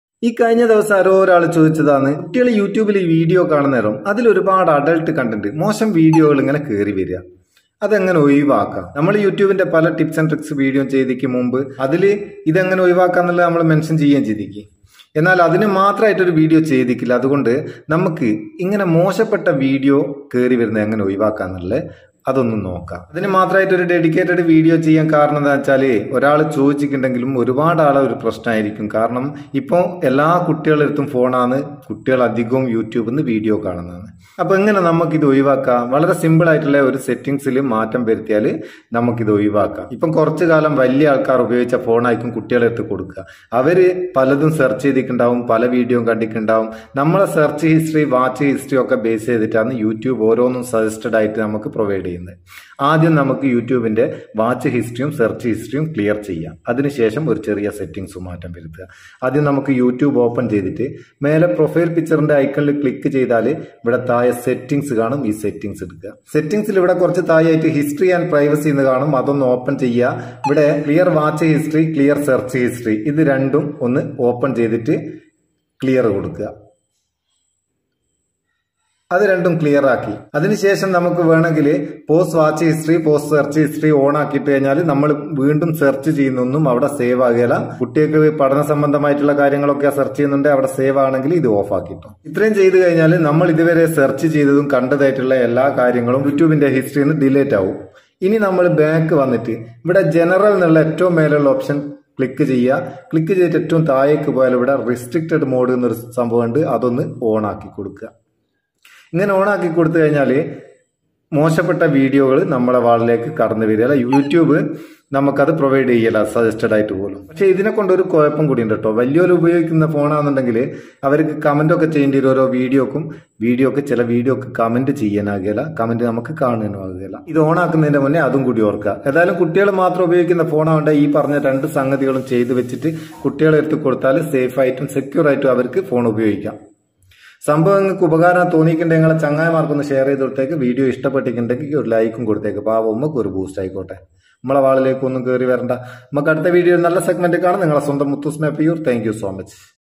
how to stop unwanted videos in YouTube
clear YouTube watch history,
clear YouTube search history
and change single settings
കൂടുതൽ അറിയാൻ ഈ ലിങ്കിൽ ക്ലിക്ക് ചെയ്യുക